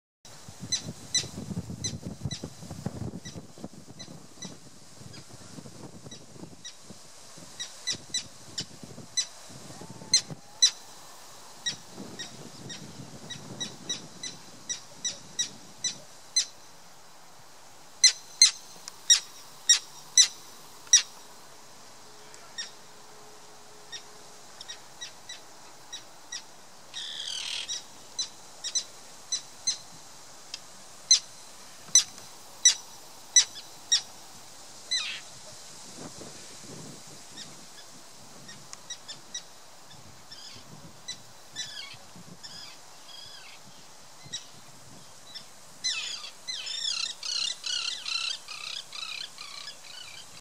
Речные крачки защищают птенцов.
Июнь 2016 года, остров Татышев на Енисее в Красноярске.